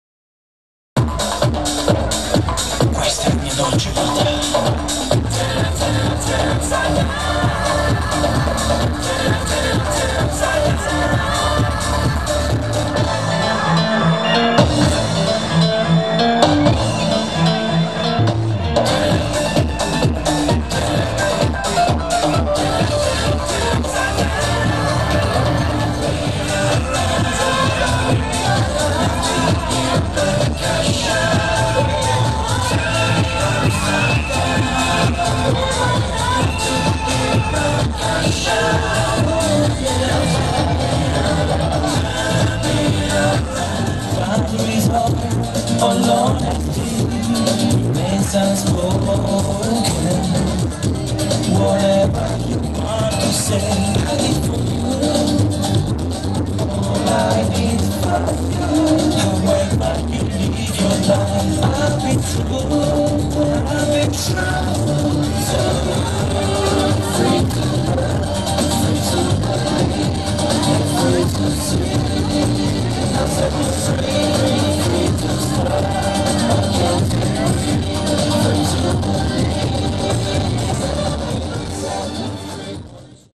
Lorenz at Belfast pride party.
Lorenz in Belfast performing at this years gay pride party.